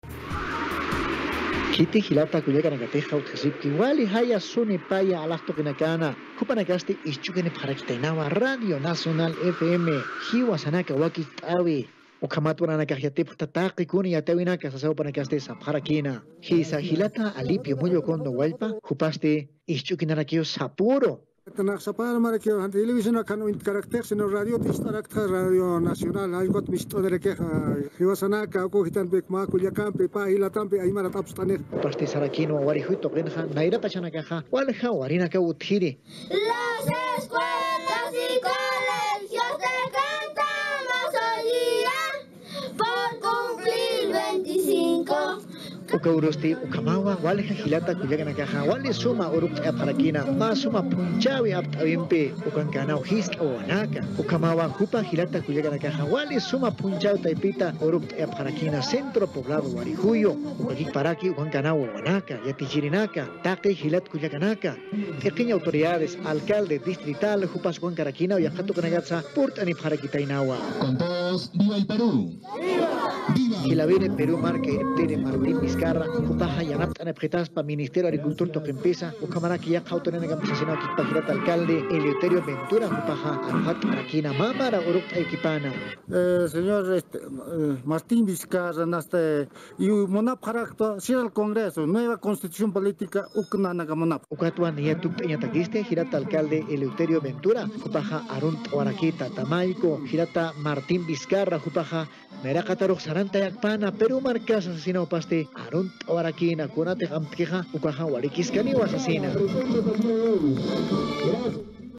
Pata pampa tuqina utjasirinakasti TV Perú ukhamasa Nacional FM taypita aski...
Pata pampa tuqina utjasirinakasti TV Perú ukhamasa Nacional FM taypita aski yatiyawi katuqapxi. Jilpacha yatxatapxañani.